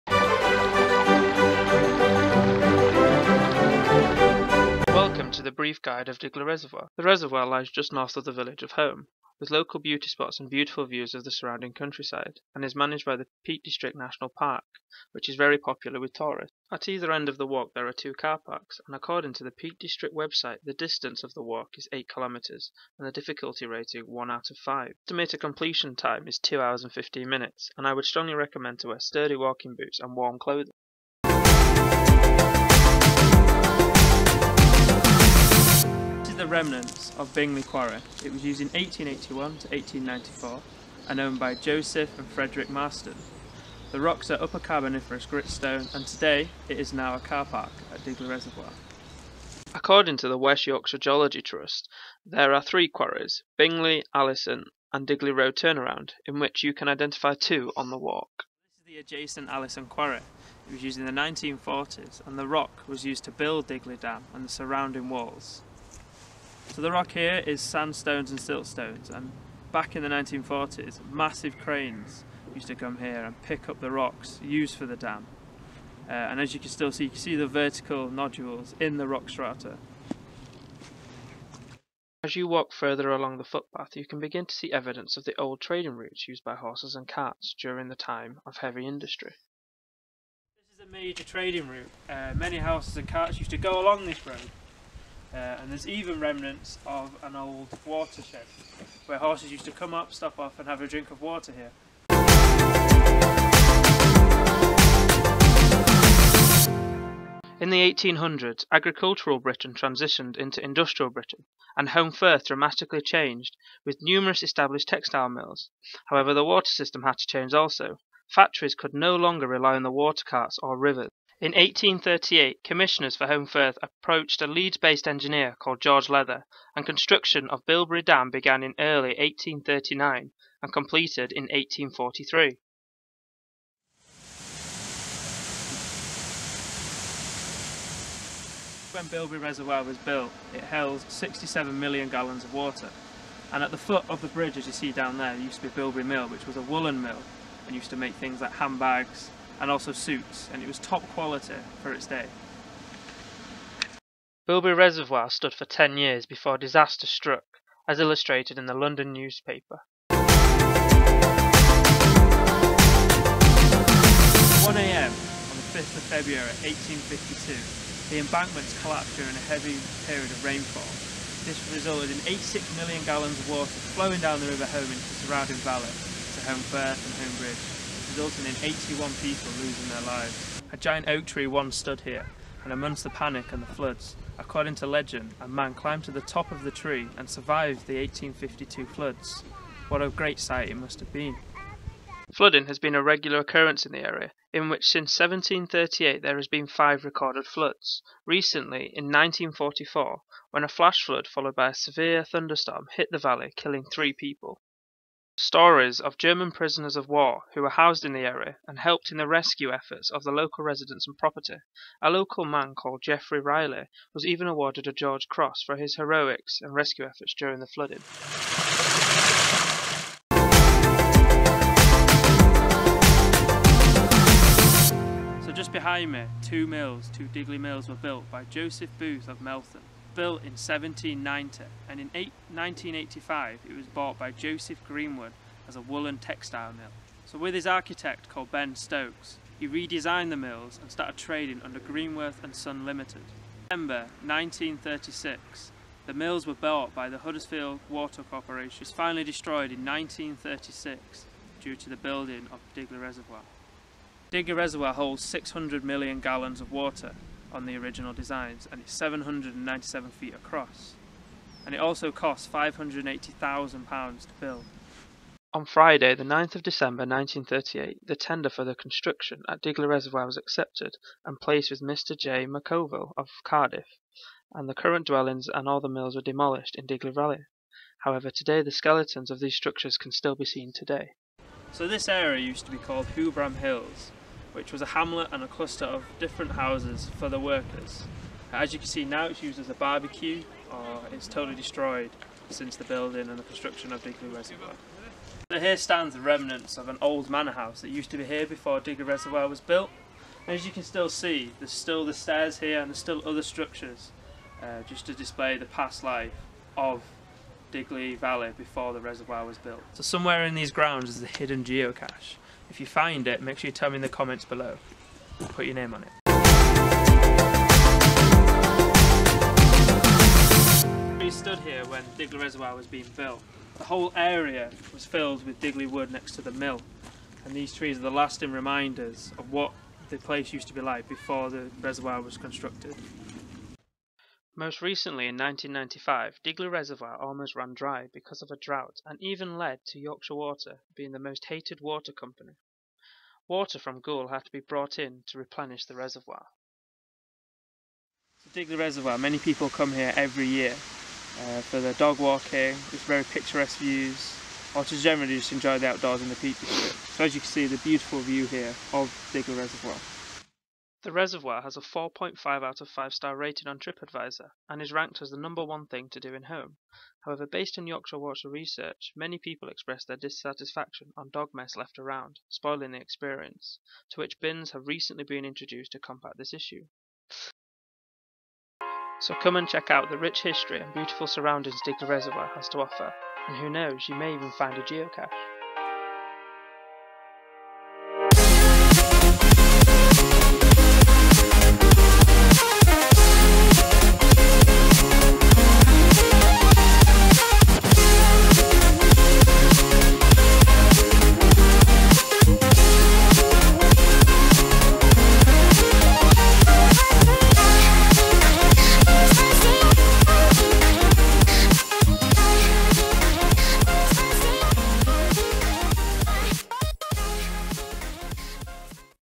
Digley and Bilberry Reservoirs Documentary
I have always been interested in documentary style filming so I have spent the past few weeks researching, filming and editing a mini documentary on Digley Reservoir.